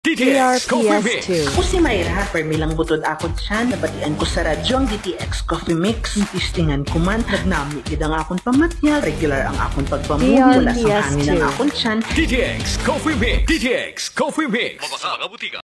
Clinica DTX Coffee Mix Radio Ad Q4 2022-2023 16s (Hiligaynon Testimonial Version 4)
Ito po ang ika-apat na bersyon ng Q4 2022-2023 Radio Ad ng Clinica DTX Coffee Mix (na base sa dialektong Hiligaynon), mula sa may-gawa ng Clinica DTX 500! Mapalit sa inyong mga botika.

DRPS Two 3290: Clinica DTX Coffee Mix Radio Ad Q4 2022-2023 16s (Hiligaynon Testimonial Version 4)